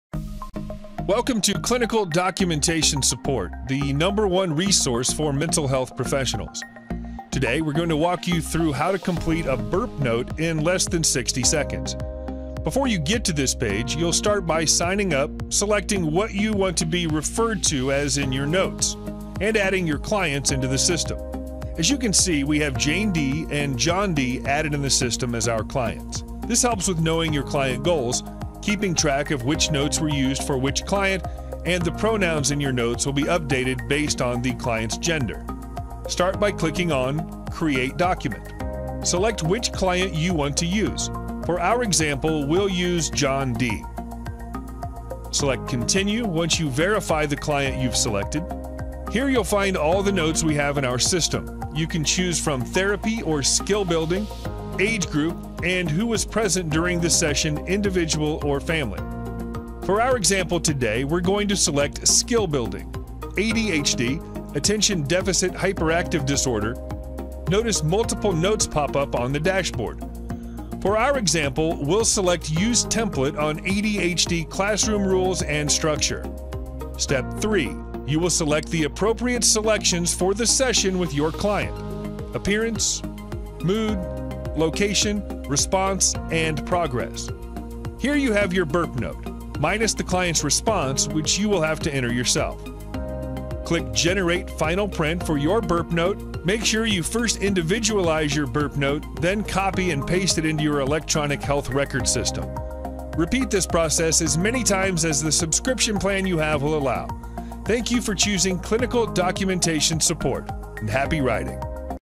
How to create a BIRP note in 60 seconds (CD Support)
In this video you will learn how to use CD support progress note generator to create a BIRP note in under 60 seconds.